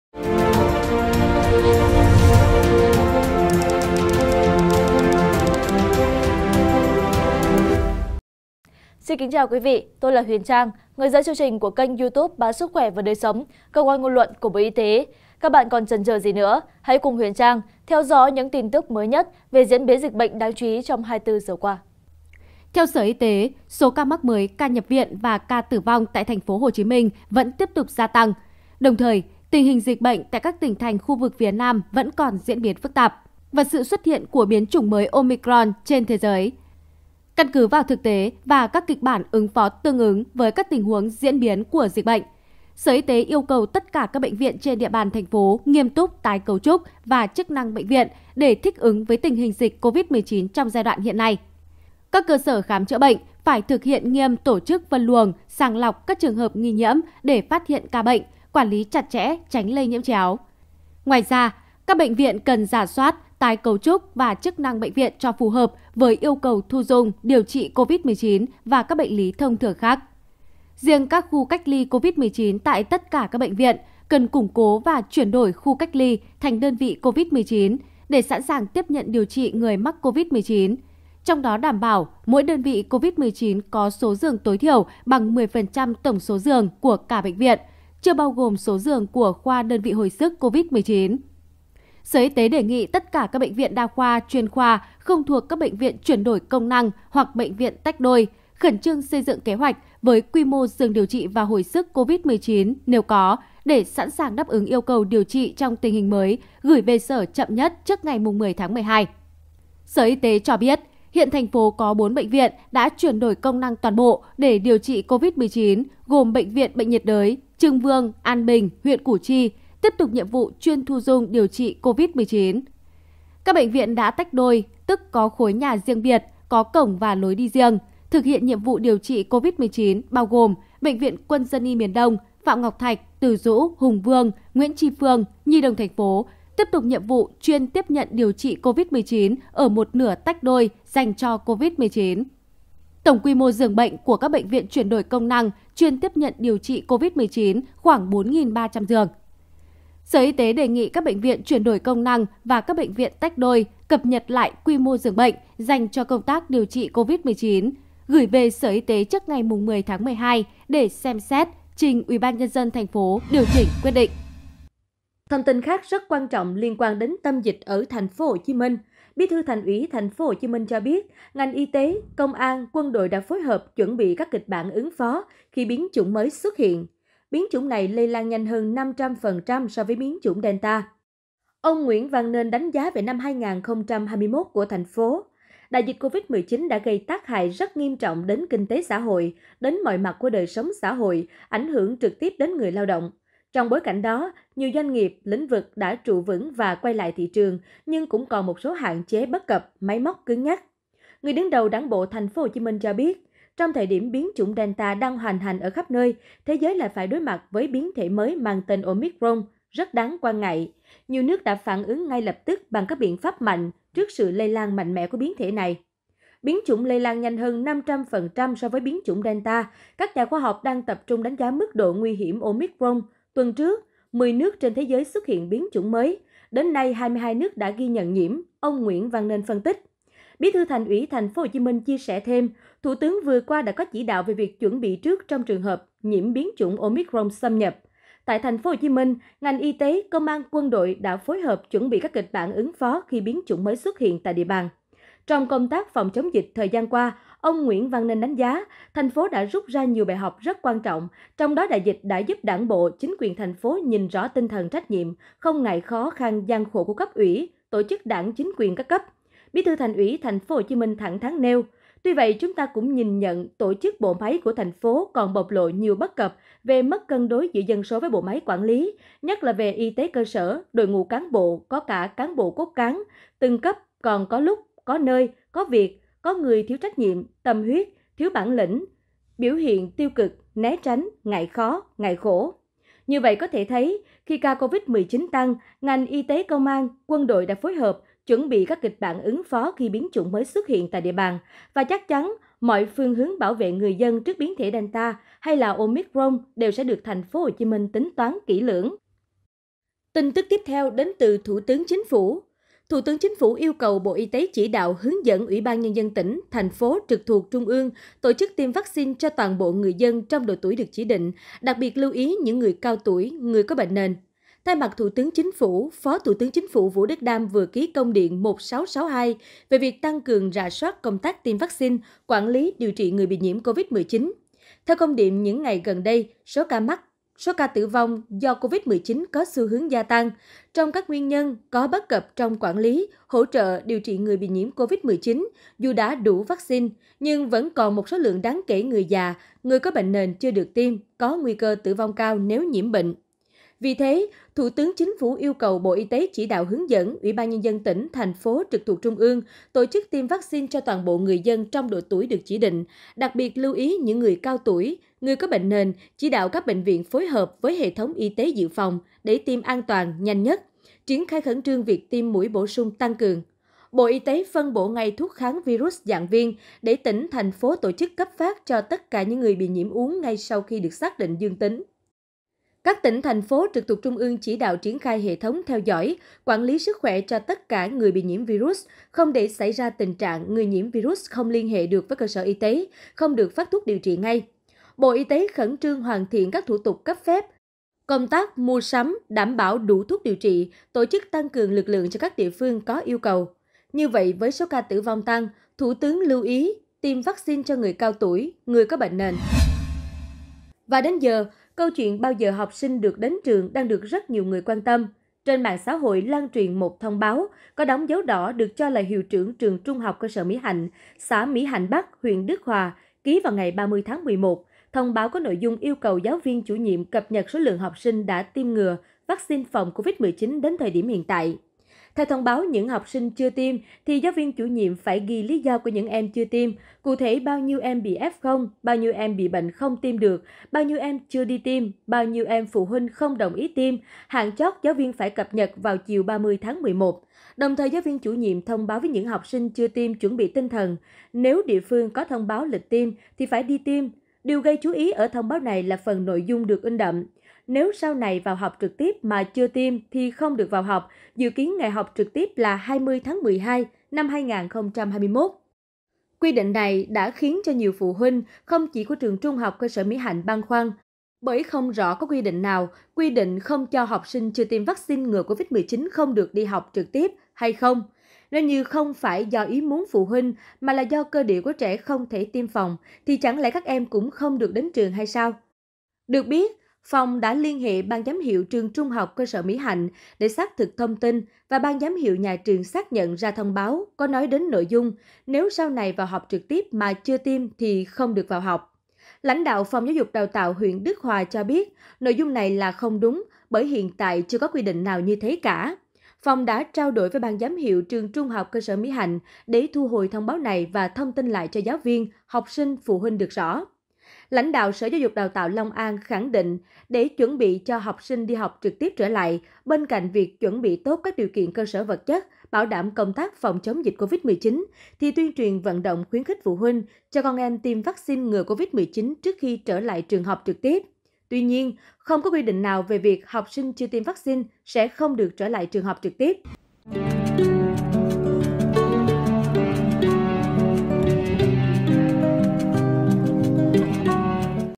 Ca Nhập Viện Và Tử Vong Do Covid-19 Tăng, Sở Y Tế Yêu Cầu Các Bệnh Viện Sẵn Sàng Đón F0 | SKĐS
SKĐS | Khi ca F0 ở TP. HCM tăng vọt, ngay lập tức TP.HCM ra văn bản khẩn yêu cầu bệnh viện sẵn sàng tiếp nhận bệnh nhân COVID-19. Cùng với đó, Sở Y tế yêu cầu các cơ sở khám chữa bệnh phải thực hiện nghiêm tổ chức phân luồng, sàng lọc các trường hợp nghi nhiễm để phát hiện ca bệnh, quản lý chặt chẽ, tránh lây nhiễm chéo. Thực hiện rà soát, tái cấu trúc và chức năng bệnh viện cho phù hợp với yêu cầu thu dung, điều trị COVID-19 và các bệnh lý thông thường khác.

Mời quý vị và các bạn xem thêm: Kết Luận Đầu Tiên Về Biến Thể Omicron: Gây Bệnh Nhẹ Hơn, Tỷ Lệ Tử Vong Có Thể Thấp Hơn